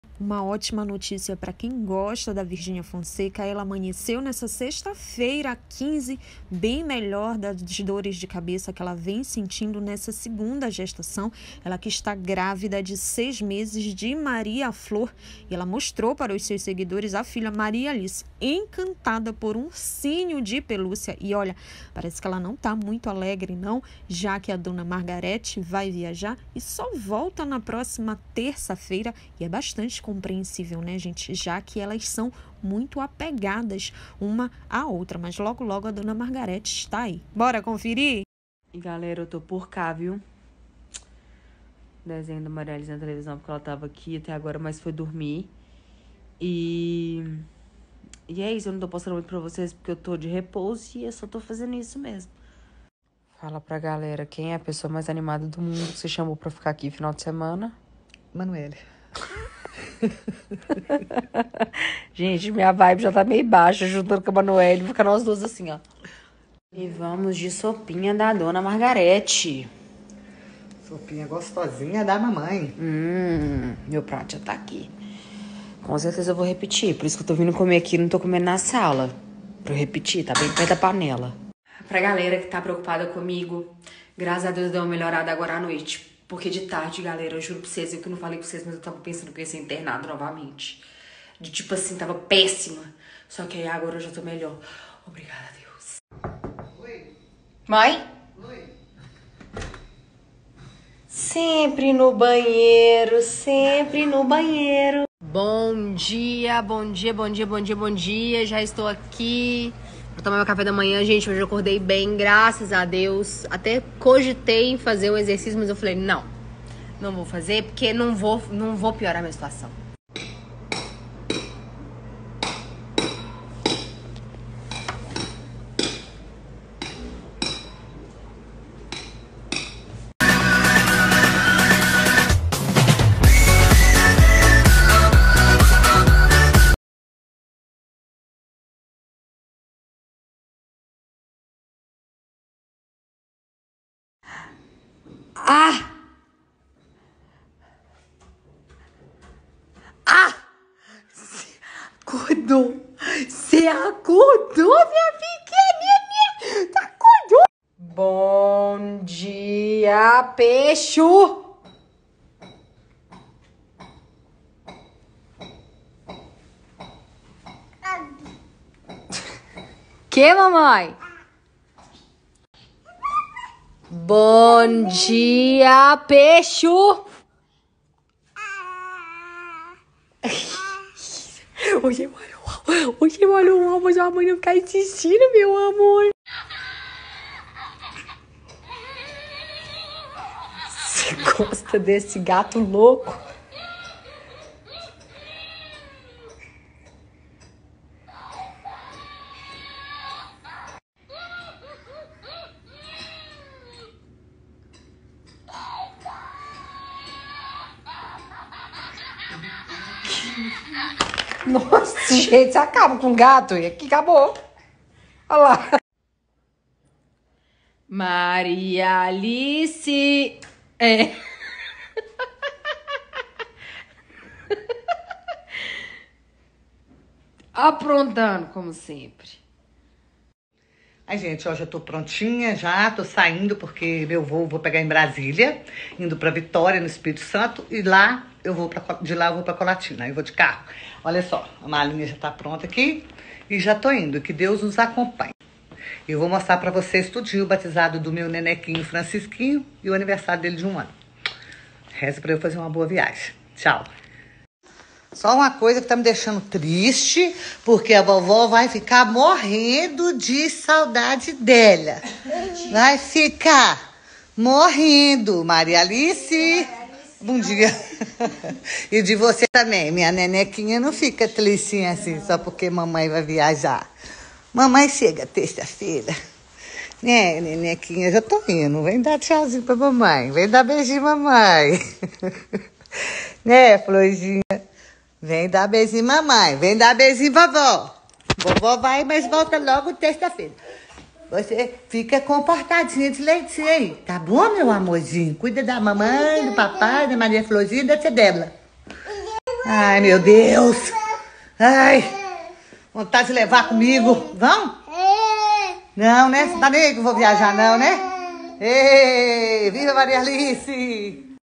Virgínia Fonseca FICA TRISTE com a MÃE/ Maria Alice se ENCANTA com GATINHO
Nessa sexta-feira 15, Virgínia Fonseca revelou aos seus seguidores que está bem melhor da dor de cabeça que vem sentindo nessa segunda gravidez, Virgínia Fonseca está grávida de 6 meses de Maria Flor. E com a viagem de dona Margareth, Virgínia Fonseca ficou triste com a ida da mãe que só volta na próxima terça-feira.